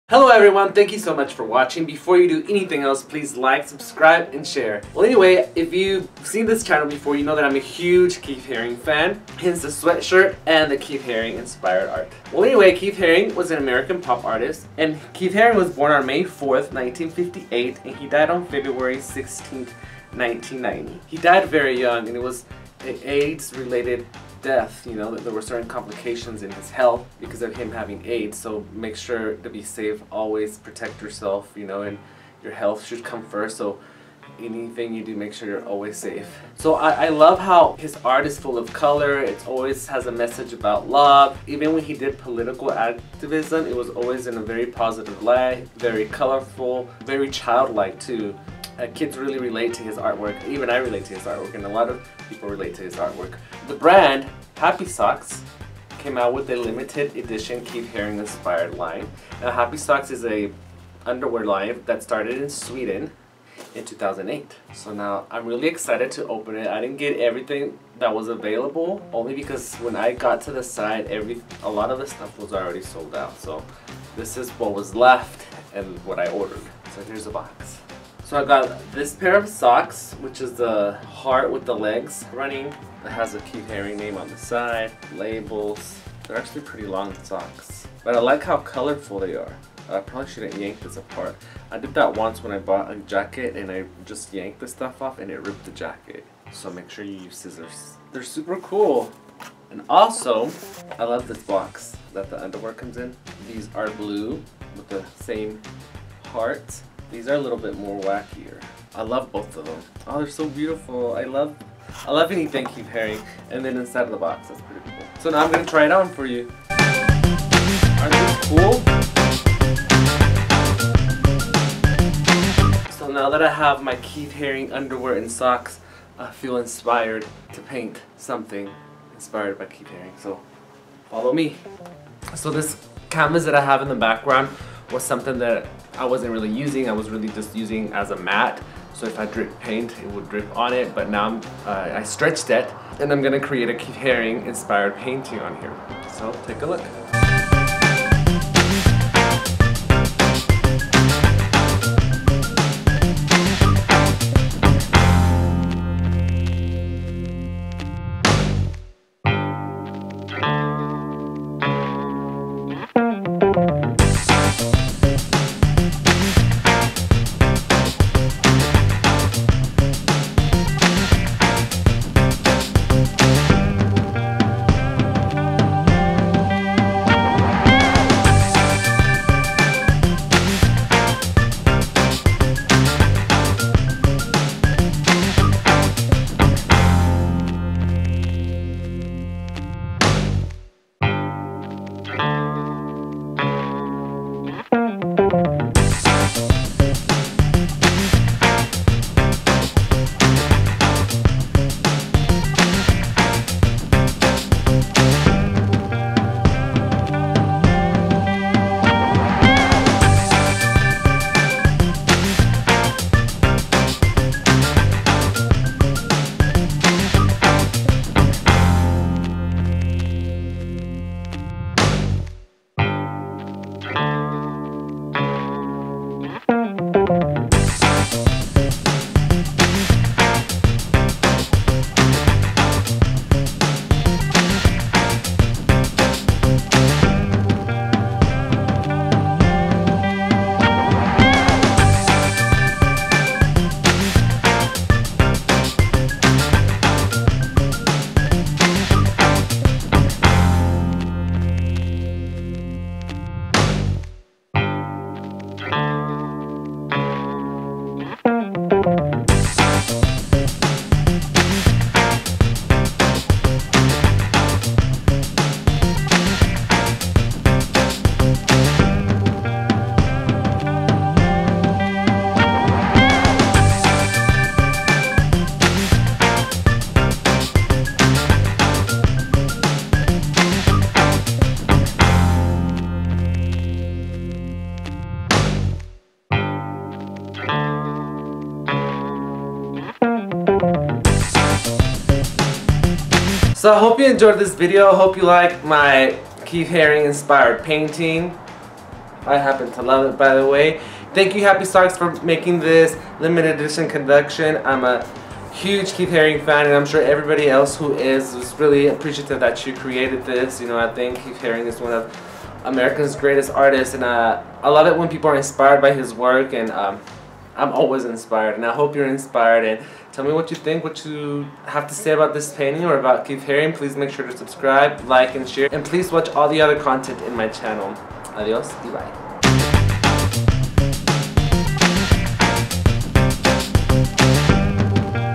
Keith Haring Happy Socks Underwear Painting
Keith Haring is American artist that I love.  Happy Socks created a limited edition Keith Haring underwear line.  Watch me review Kieth Haring underwear , and paint in Keith Haring underwear.